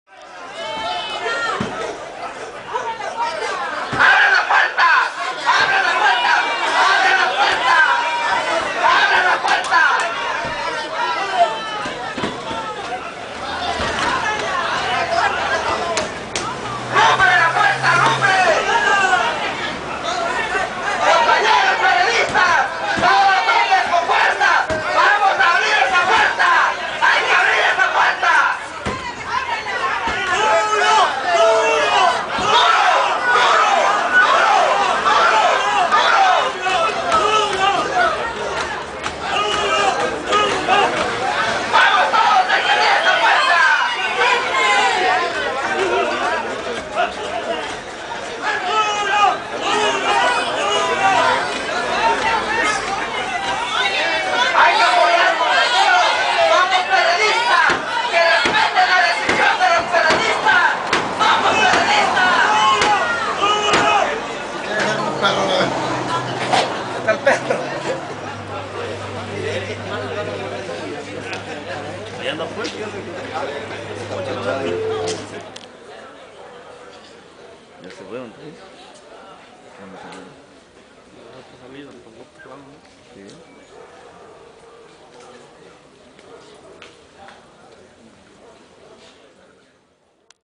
PRD La simulacion del Consejo en Chilpancingo.mp4
Protesta de perredistas excluidos en el salón Los Cedros en Chilpancingo Guerrero el 2 de abril de 2012 a las 02:20 am. En donde después de abrir las puertas no se encontró ninguna sesión de Consejo, lo cual desmiente las declaraciones de que si se llevó acabo el acuerdo de asignación de candidaturas.